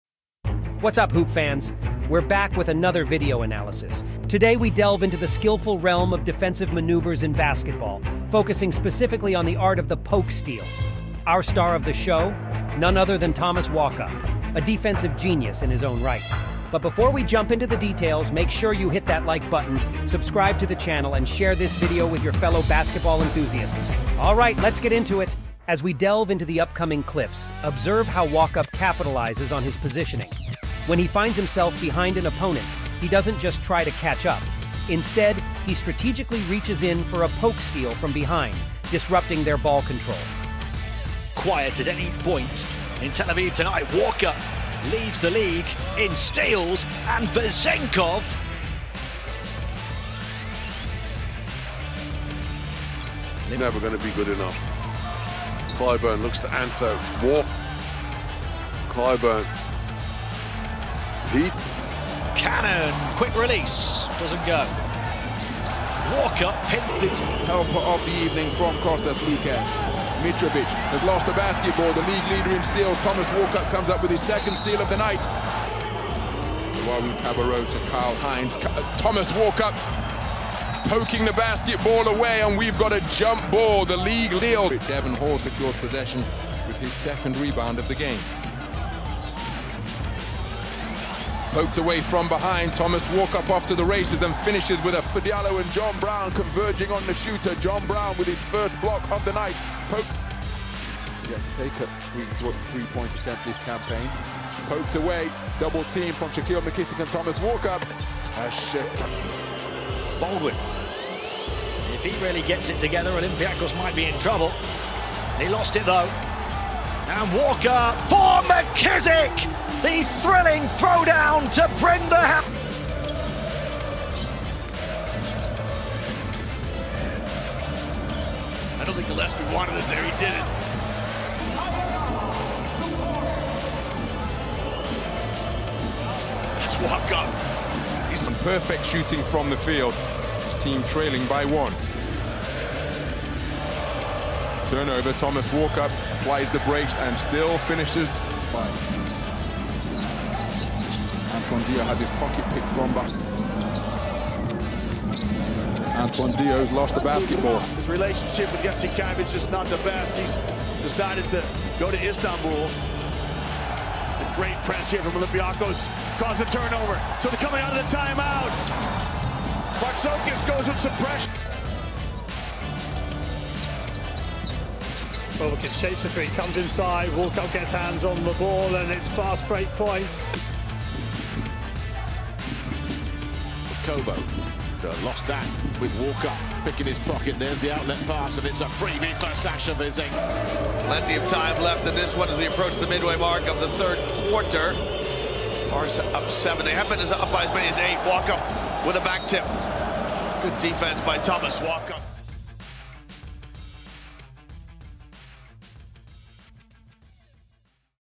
The Art of Stealth: Unveiling Thomas Walkup's Masterful Poke Steals
Dive into the mesmerising world of defensive basketball with us as we spotlight the exceptional Thomas Walkup. Renowned for his skilful 'poke steals', Walkup's behind-the-scenes action often leaves opponents bewildered and fans astounded. Watch and learn from the best as we break down his technique, timing, and execution. Don't miss this chance to witness the artistry behind this game-changing manoeuvre.

Additionally, we utilise high-quality game footage from WSC Sports to bring you the best content possible.